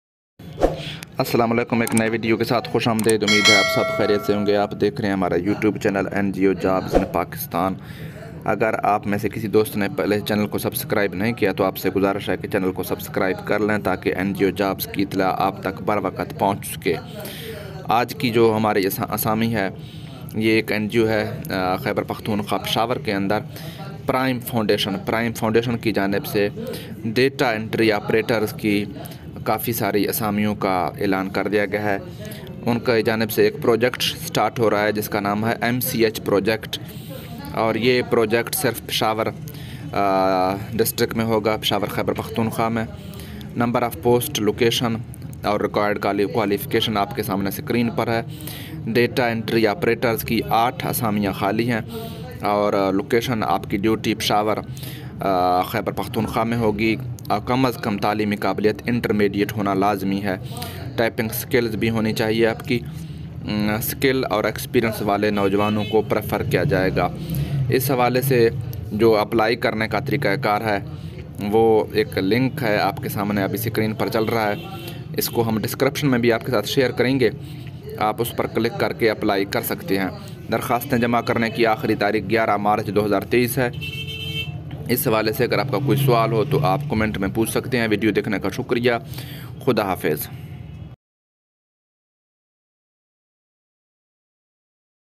Prime Foundation Data entry operators Jobs 2023 | KPK NGO Jobs 2023 apply online
Prime Foundation Data entry operators Jobs 2023 are announced.You can apply KPK NGO Jobs 2023 online on the web address.
Data entry operator jobs in KPK are 8 in number.